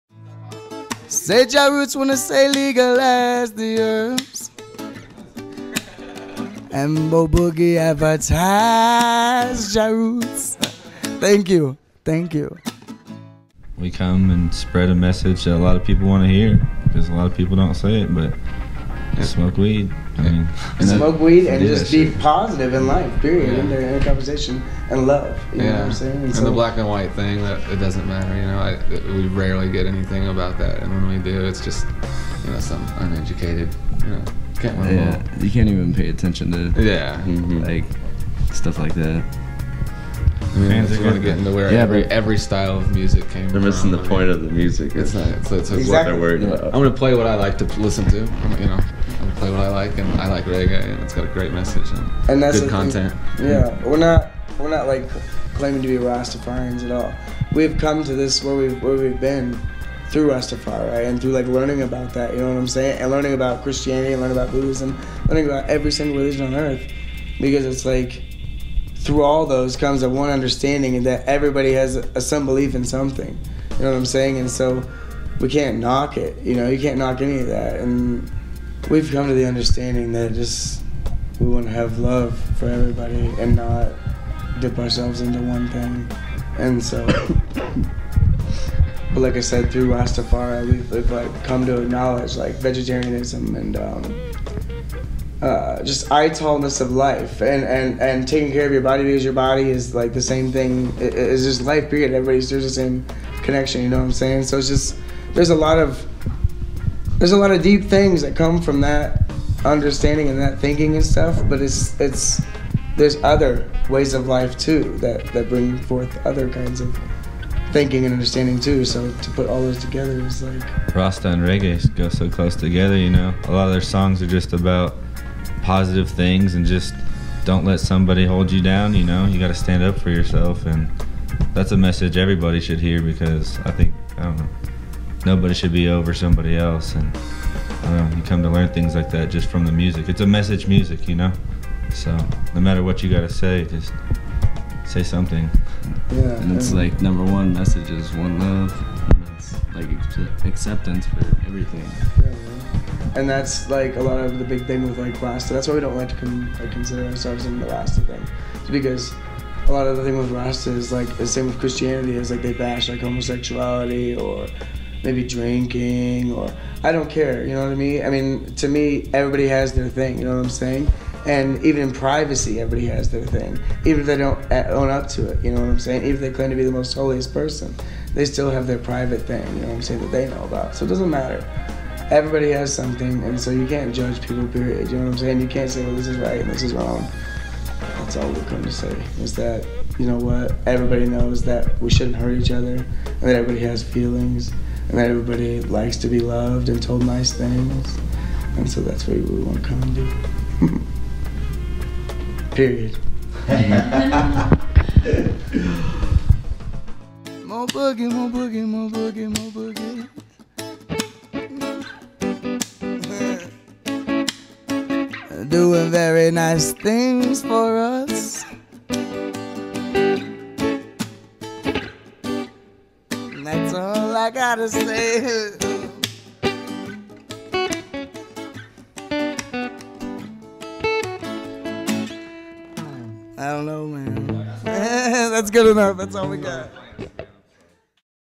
Jah Roots "Rasta" - webisode at the MoBoogie Loft 4-26-08
webisode: Jah Roots talks about the influence that Rastafarian culture has had on their music and how they do not claim to be Rastafarian but have developed their own system of beliefs based on acceptance and understanding of all cultures.